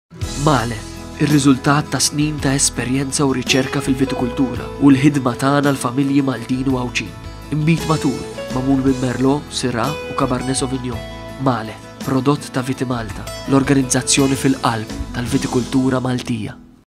Vitimalta: Maleth wine by Maltese Viticulture Farmers Producer Organisation
Vitimalta is the largest Producer Organisation in the Maltese Islands. It is composed of over 300 viticultural farmers with vineyards all over Malta and Gozo. Vitimalta's produce represents around 85% of all local wines. Furthermore Vitimalta also produces its own wines; the Maleth and the Auctoritas range.

Maleth is a blend of Merlot, Syrah and Cabernet Sauvignon grapes with intensive aromas of red forest fruits and cherry.